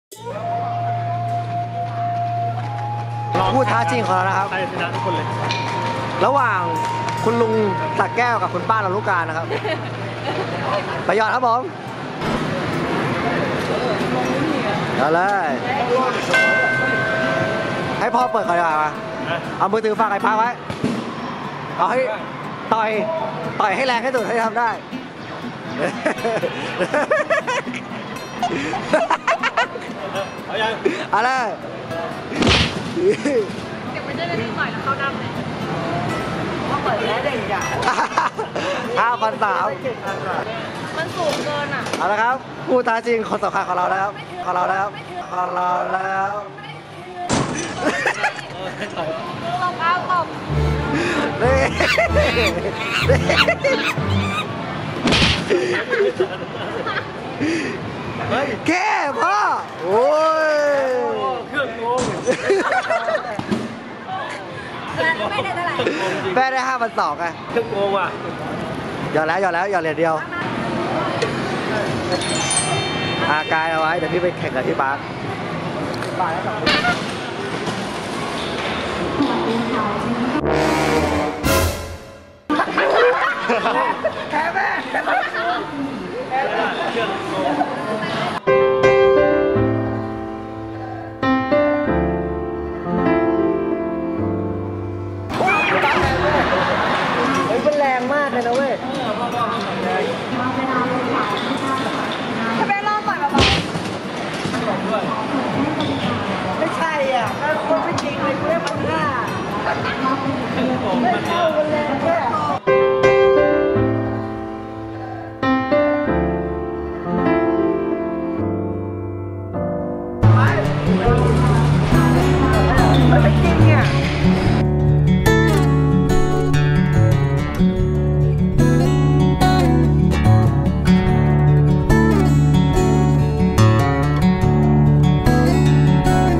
แข่งต่อยเกมตู้งานนี้คนกากสุดจะเป็นใคร!!! [Parkmalody Funny]
เมื่อปาร์ค กับ ครอบครัว แข่งต่อยเกมที่วัดแรงต่อย
ใครจะกากสุด ใครจะเก่งสุด ไปดู!

กล้อง : panasonic lumix g85
โปรแกรม : final cut pro x